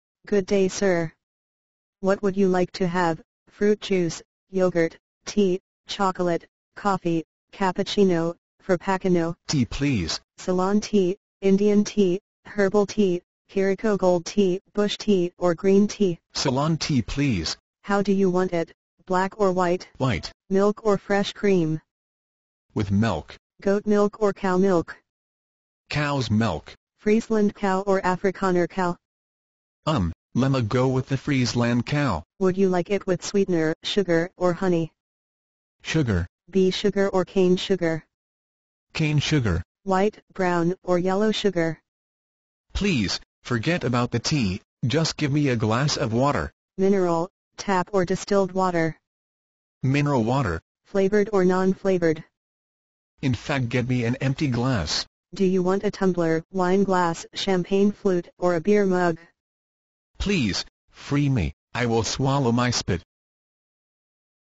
The maid & guest
What you get when you visit I rich man house. Funny animation